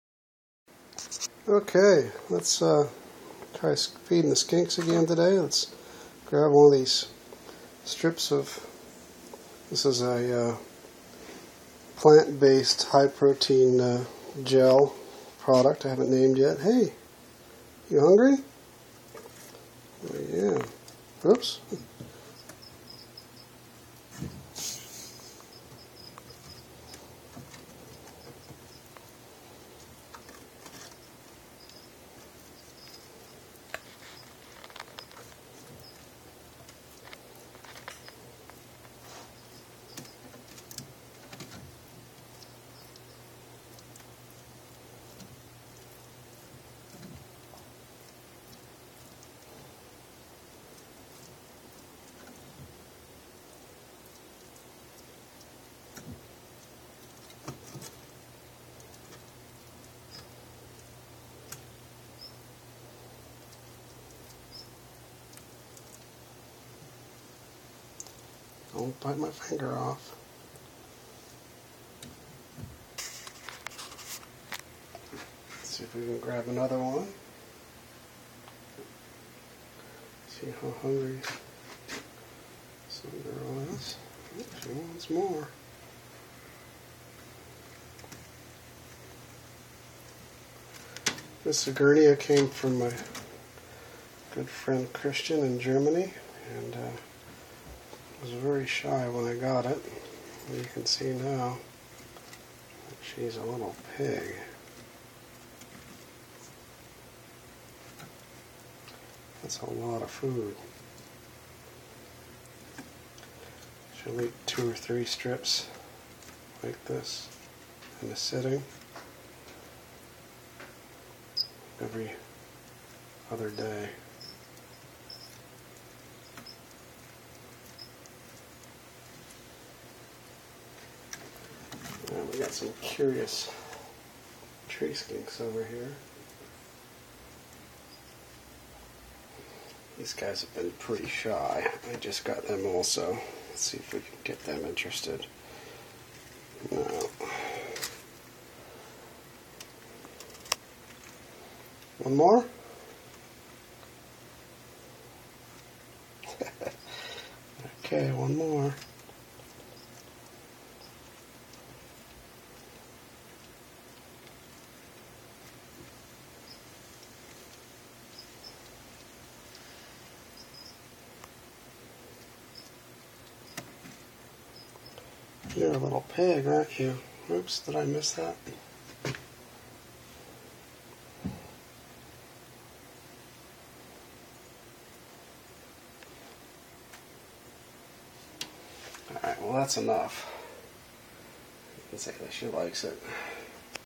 Egernia eating Repashy
Testing a new medium protein level omnivore gel (30% protein) with the Egernia stokesi, Egernia striolata, and Egernia depressa. This girl is a pig and seems to prefer it over the formula with animal proteins. Next, I will try a meat replacement formula that will be 50% protein vegan formula.